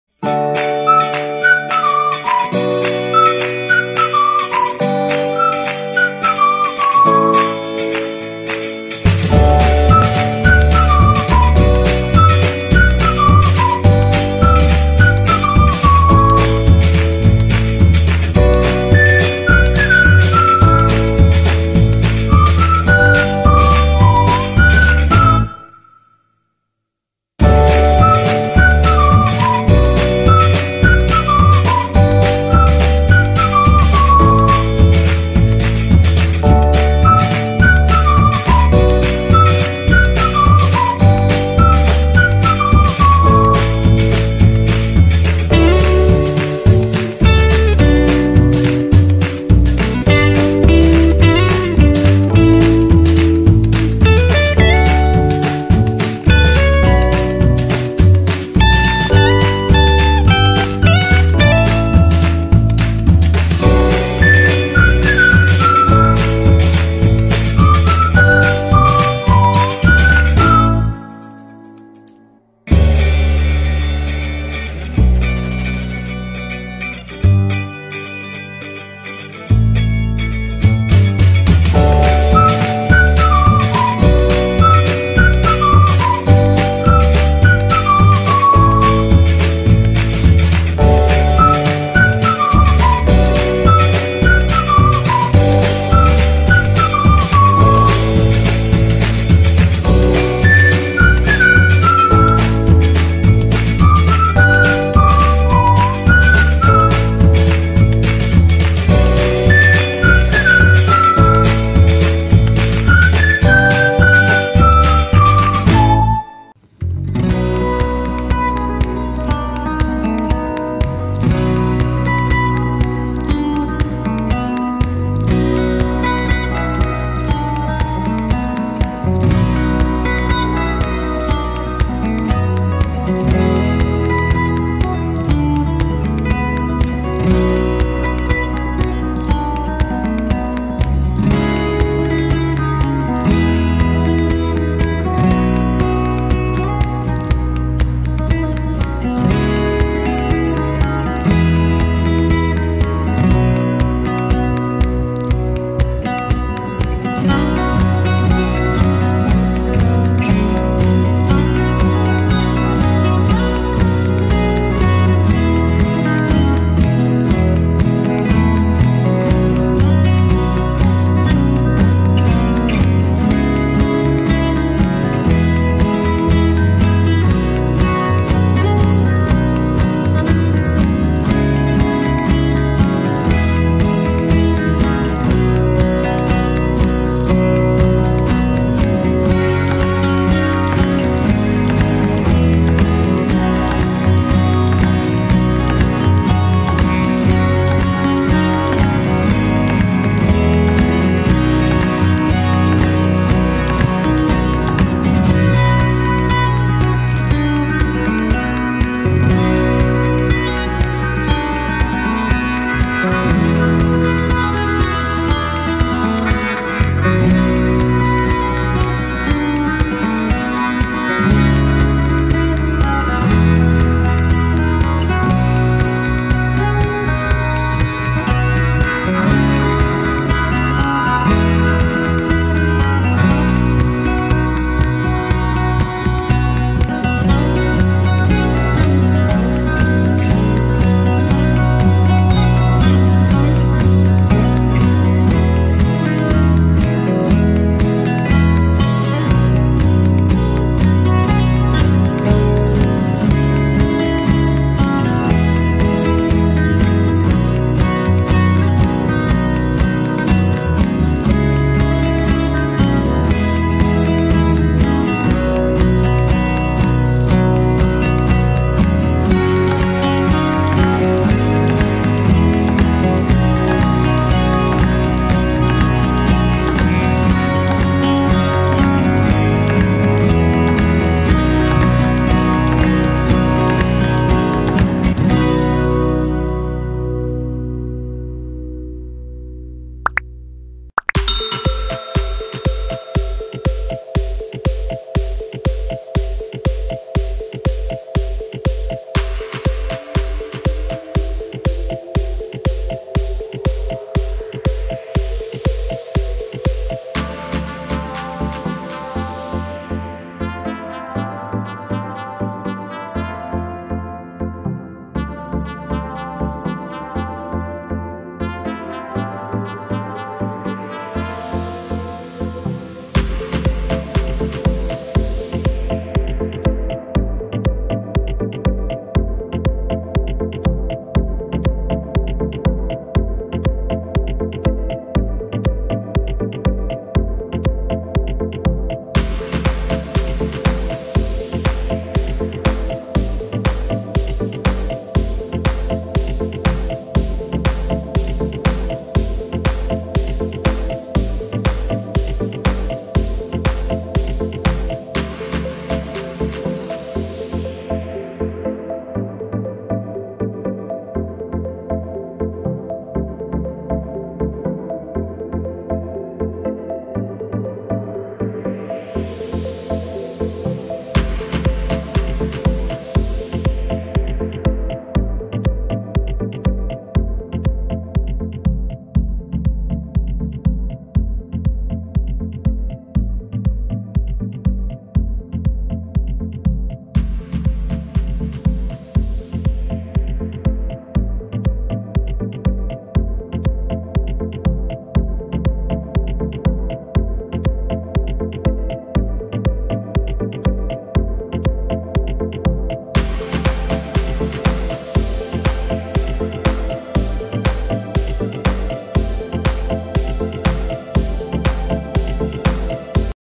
DIY remote control
We tried to show you how the control panel is made for our crafts, now you can repeat it yourself if you need it